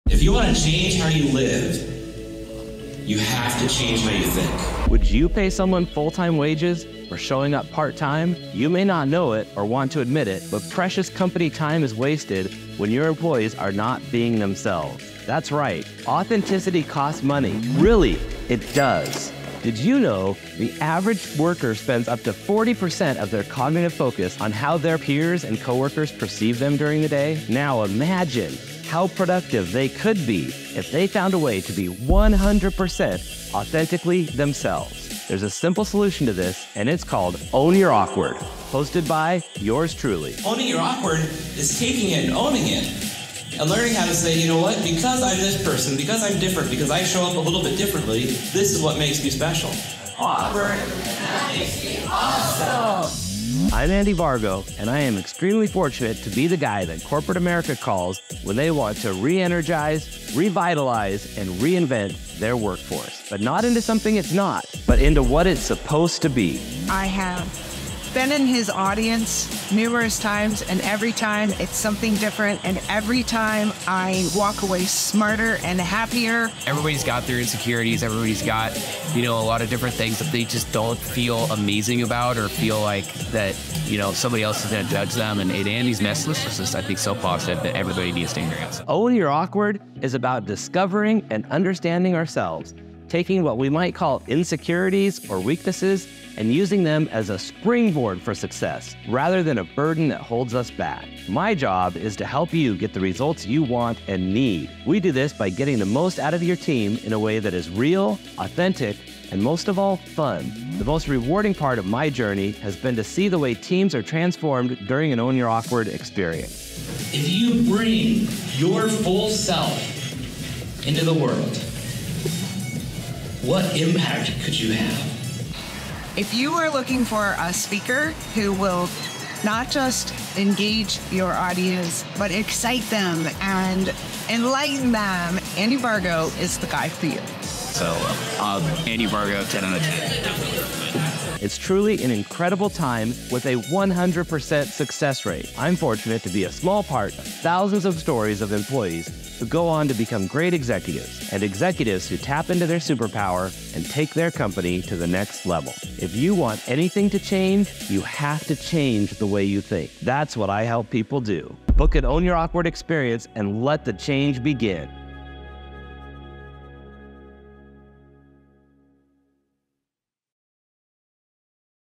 Andy Vargo Speaker Reel: Own Your Awkward
Andy Vargo is the guy for you when it comes to engaging, empowering, and entertaining your audience. If you want anything to change, you have to change the way you think. From executive leadership retreats, to employee development days, to keynote speaking, Andy Vargo is who you need.